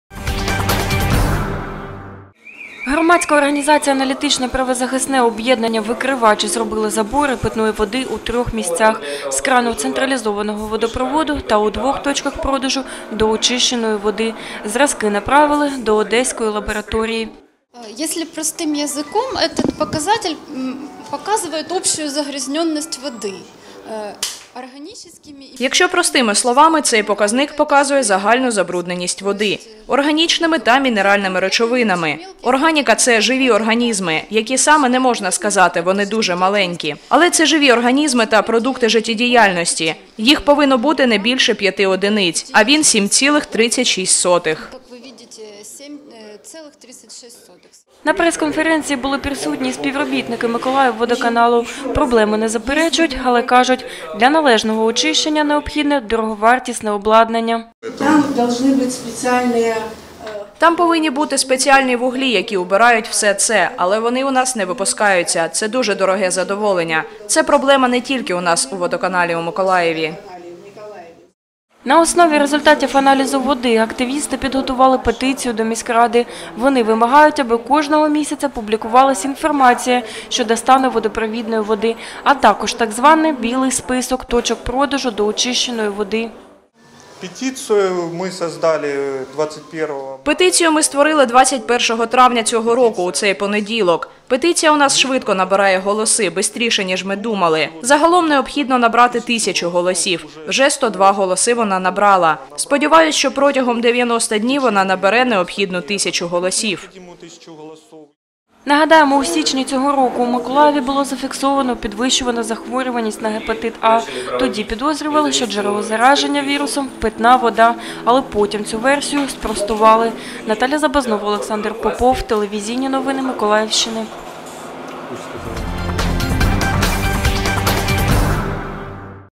У Миколаєві питна вода не відповідає вимогам державних санітарних норм
- У Миколаєві питна вода не відповідає вимогам державних санітарних норм, - про це на прес-конференції в Миколаївському кризовому медіа-центрі заявили представники ініціативної групи проекту “За безпечну воду”.